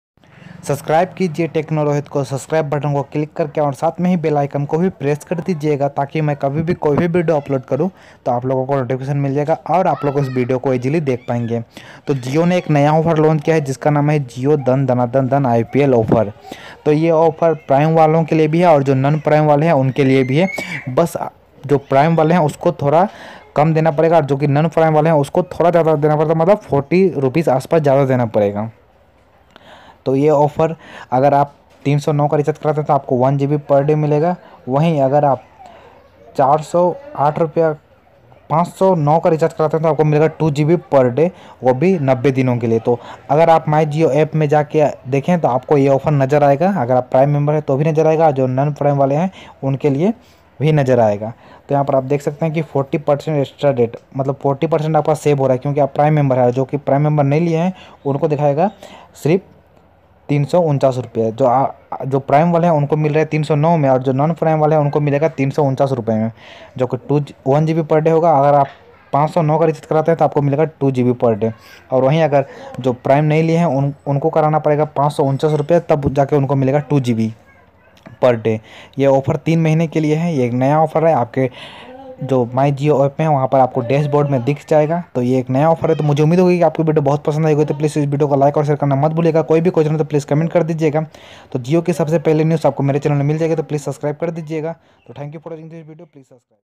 जियो धन धना धन IPL OFFER
Jio Dhan Dhana Dhan IPL Offer
Aaj maine aplogo ko jio ke naye offers ke bare me bataya hu mujhe umeed hai ki aapko meri videos pasand aaygi Please like aur share kijiye aur koi bhi questions hai to please comment kijiye